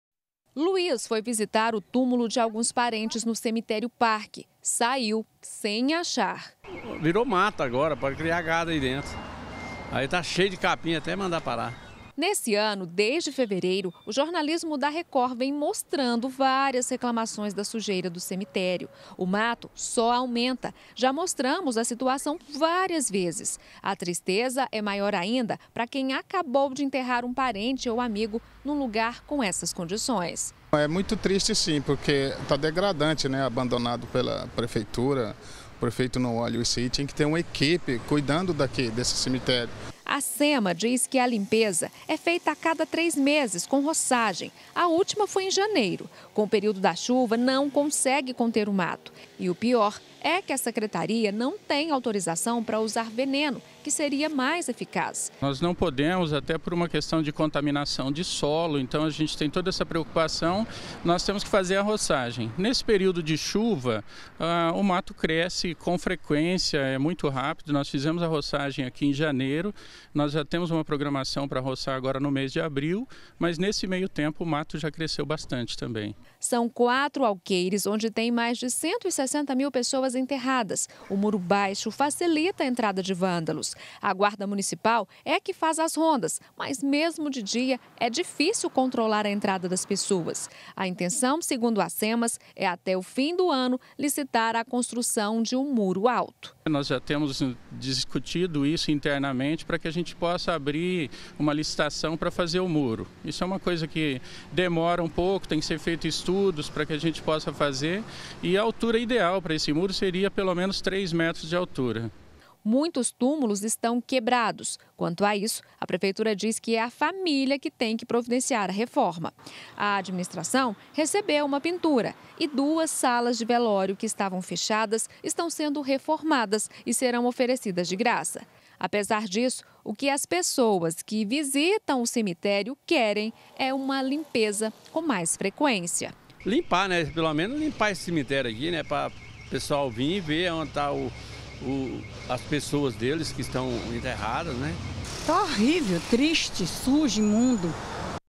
CA - Cemitério tomado pelo mato - 14-03-2018
Quem visita o cemitério parque reclama do abandono do lugar. O mato continua alto e o muro baixo facilita a entrada de vândalos.